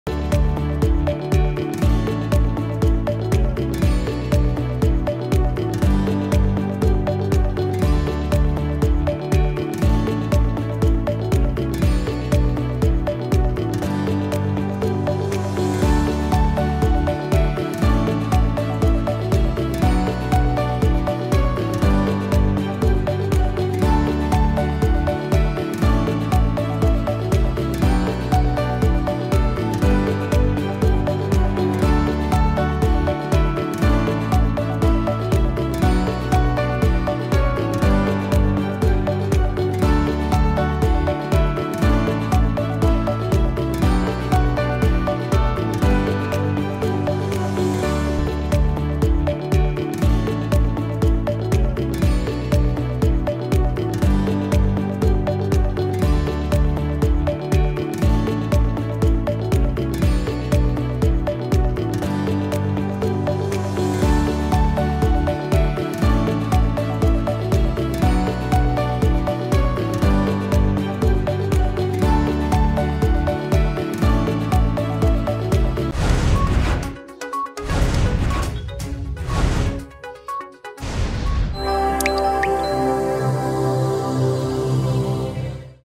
Tiranga Shikara rally held at Dal Lake in Srinagar under Har Ghar Tiranga Campaign
Tiranga Shikara rally held at Dal Lake in Srinagar under Har Ghar Tiranga Campaign. The shikara rally was flagged off by Lt Governor Manoj Sinha.